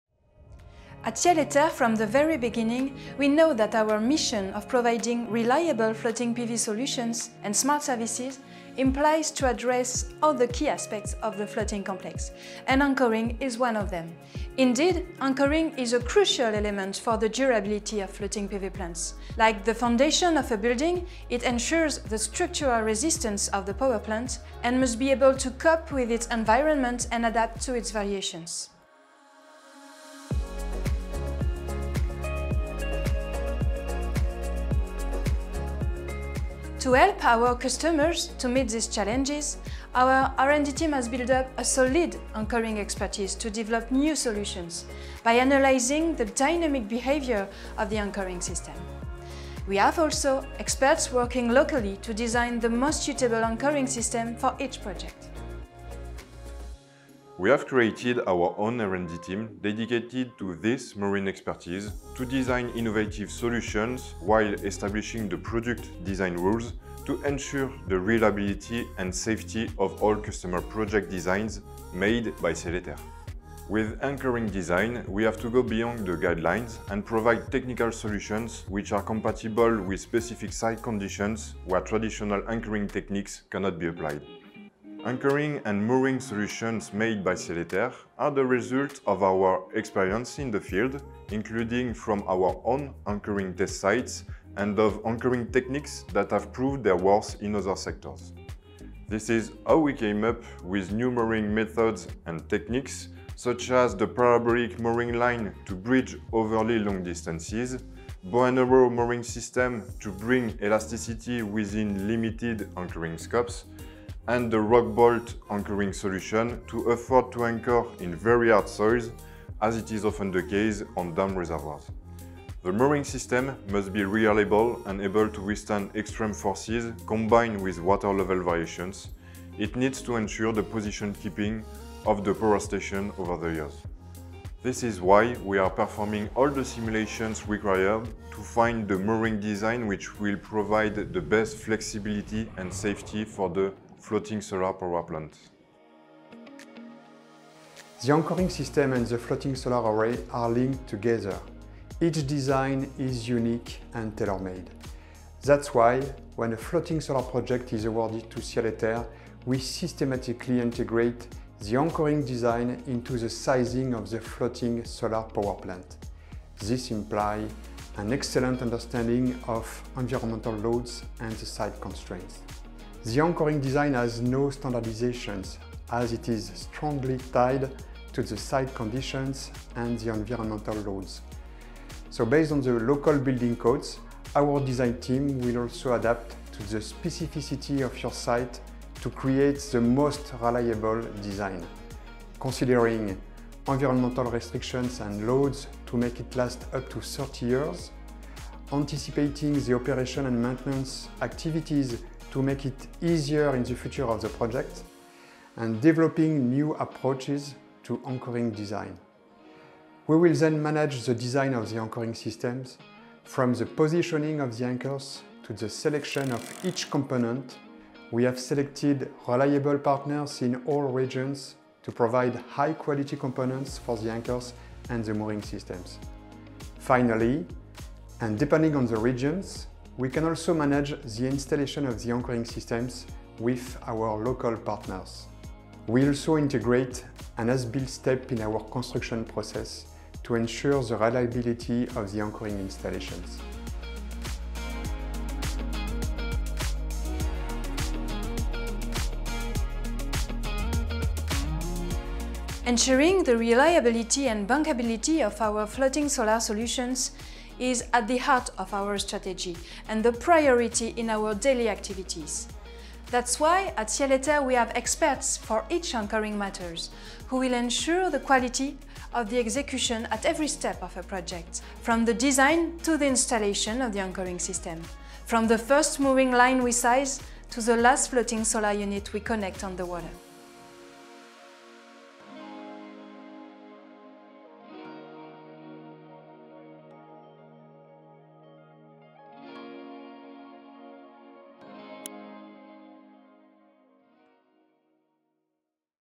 Floating solar anchoring system : Ciel & Terre’s engineering expertise for reliable FPV projects
At Ciel & Terre, we understand that the reliability of a floating solar power plant depends on a strong, durable, and site-adapted anchoring system.
In this video, discover how our R&D team develops innovative and tailored solutions to ensure structural integrity and long-term stability, even under the most extreme conditions.
To support each project, our dedicated project teams manage anchoring design, procurement, and installation locally, ensuring the most effective implementation on site. 🌊⚙️

✅ Tailored anchoring systems based on wind, water current, and bathymetry data
✅ Proven innovations: parabolic mooring lines, rock bolt anchors, bow & arrow systems
✅ Advanced simulations using Orcaflex software, validated by full-scale field tests
✅ End-to-end floating solar anchoring expertise, from engineering to on-site deployment